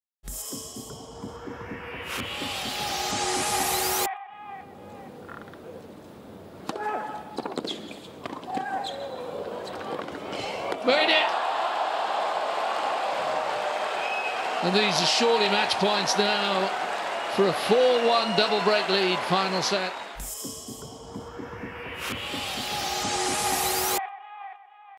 Baghdatis Sprints For Hot Shot in Dubai 2016
Marcos Baghdatis is on the run for a shot in his semi-final against Feliciano Lopez in Dubai. But will he get there in time? Watch live tennis at tennistv.com. Photo: Getty Images